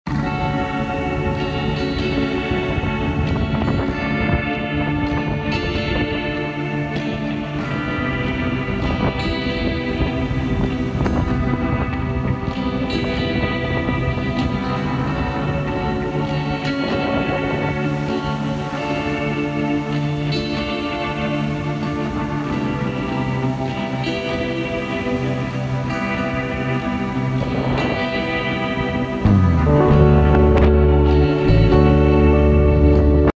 Foggy day on the Gulf
Walking the beach on creepy fog day. Florida Winter